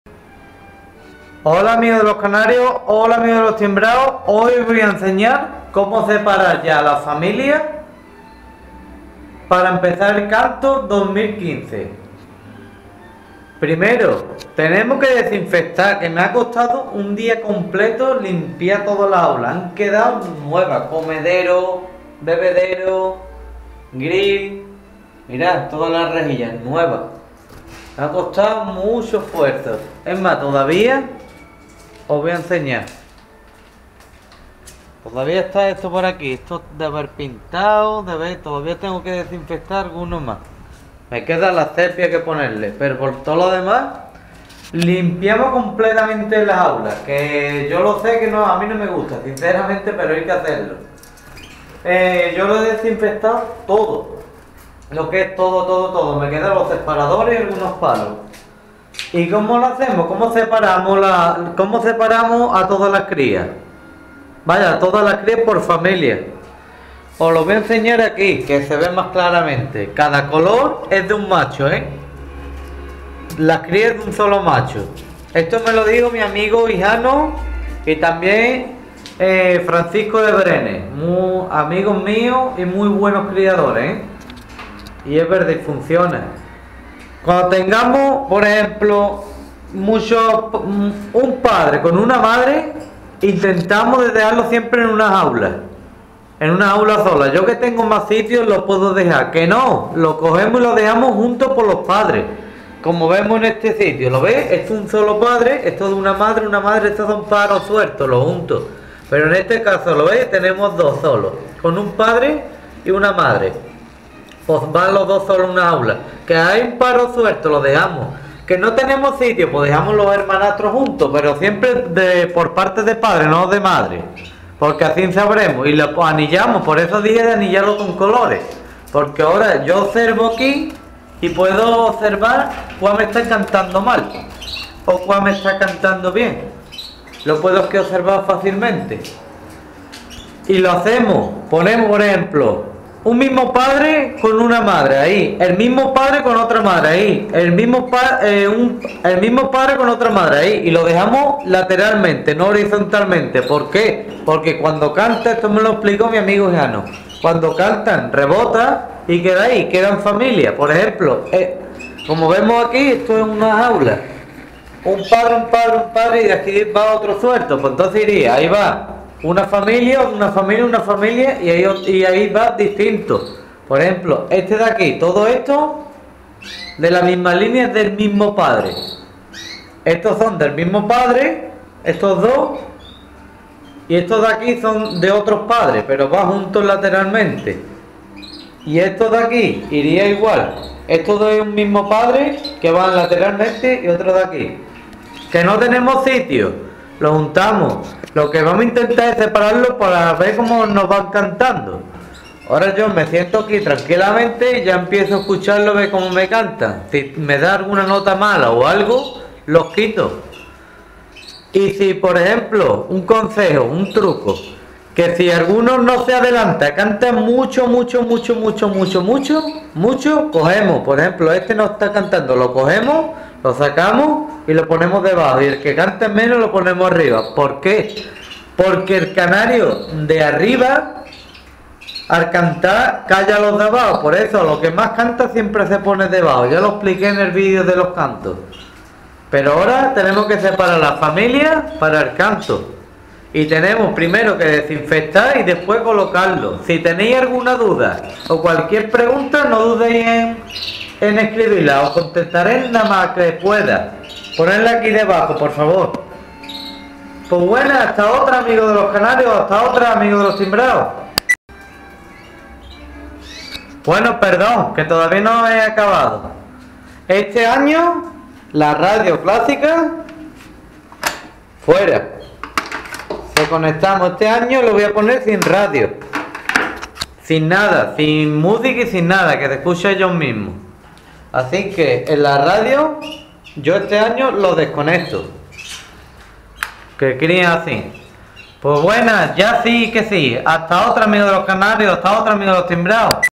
SEPARACION DE CANARIOS PARA EL CANTO (HD)
Espero que os guste como los separo, aqui os muestro como lo hago y lo haré este año se ven los canarios sanos, repare todos las jaulas y limpieza de todos los bebederos, clasificar siempre horizontalmente a los hijos del mismo padre y de distinta madres y anillas de colores para clasificar a los de la misma familia, saludos  XD